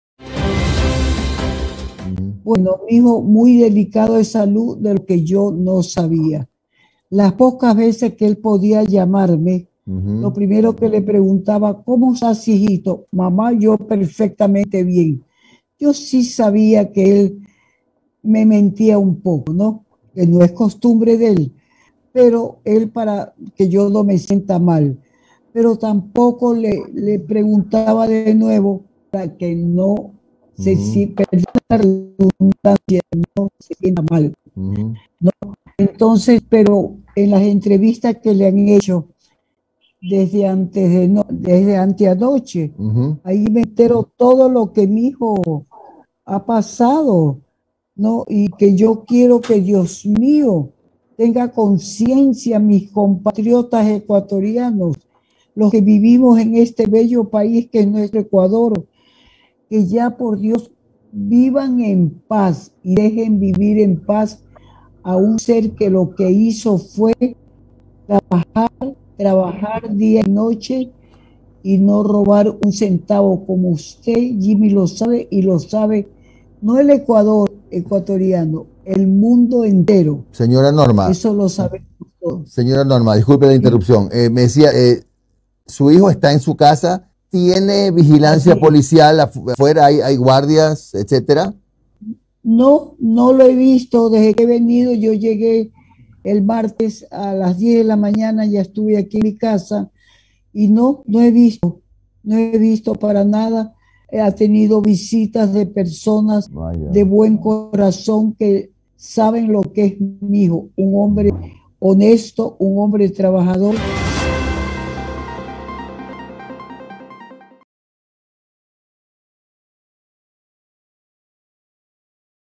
ElAmorVencióAlOdio - Doña Norma Espinel comparte su alegría por la libertad de su hijo, Jorge Glas.
Doña Norma Espinel, madre de nuestro legítimo vicepresidente Jorge Glas, comparte su alegría por la libertad de su hijo, un hombre inocente que debió salir de prisión hace 15 meses. Desde el gobierno de Moreno, se vulneraron sus derechos por persecución política, pero finalmente ElAmorVencióAlOdio.